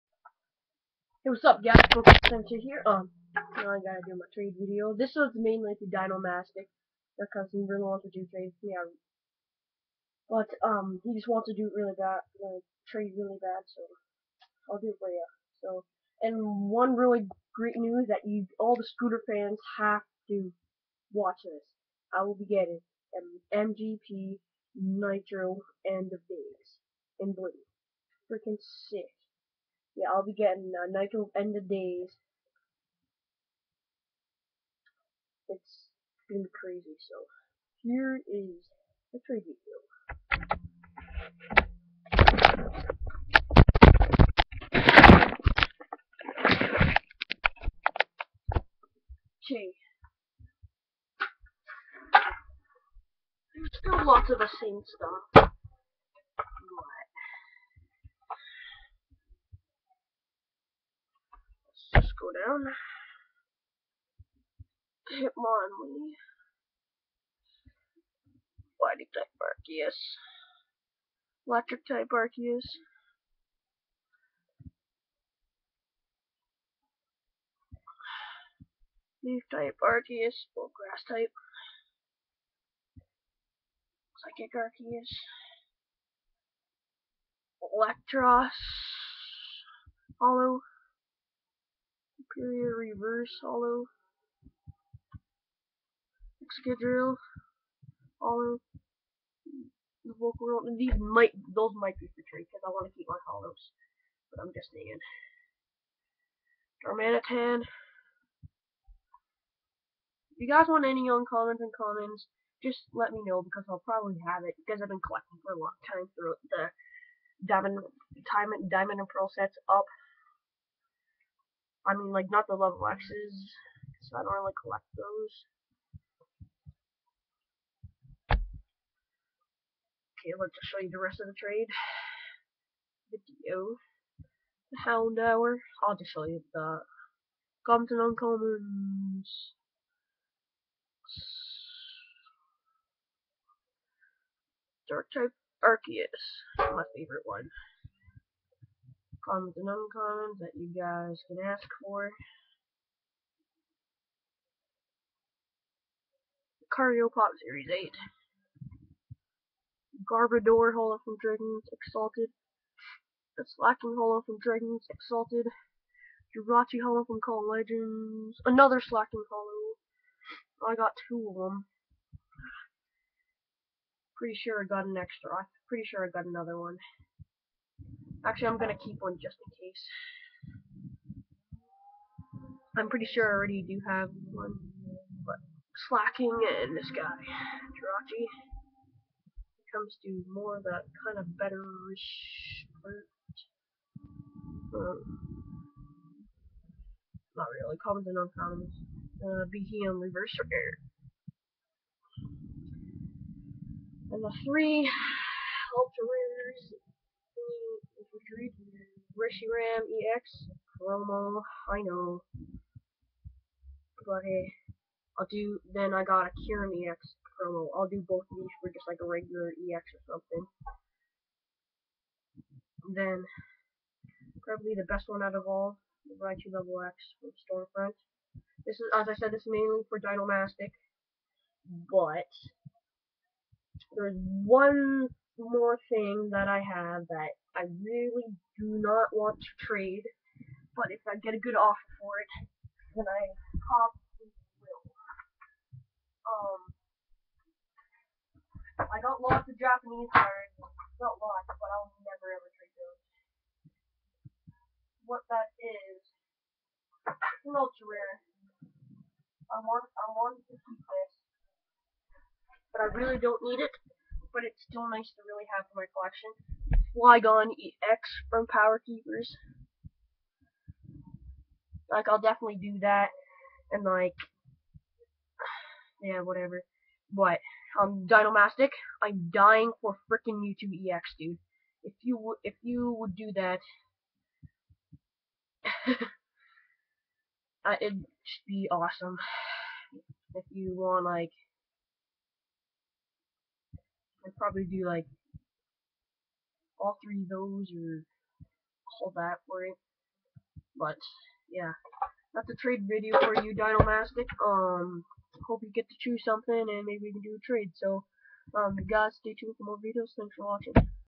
Updated trade video! + some awesome new about my new scooter that im getting
These are my newer cards for trade as i said in the video this trade video is mainley for Dinomastic